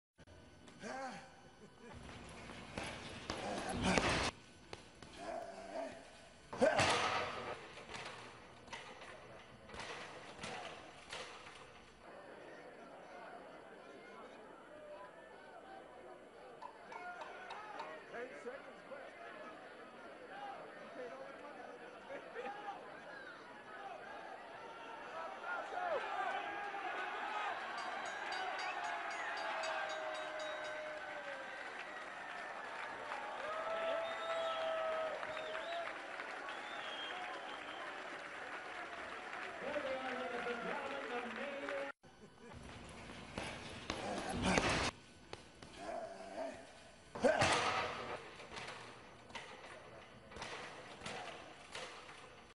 Boxing great Joe Frazier dies
the former heavyweight champion who handed Muhammad Ali his first defeat yet had to live forever in his shadow, died Monday night after a brief final fight with liver cancer. He was 67.